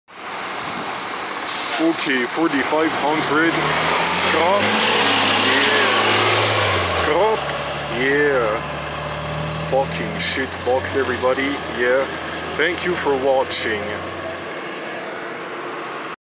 OC bus #4500 a 2007 New Flyer D40i low floor bus (SCRAP!) @ the OC Hurdman Transitway station
Film today by myself everybody! Happy friday everybody!✌️🙏🙏🙏🙌🙌🙌✊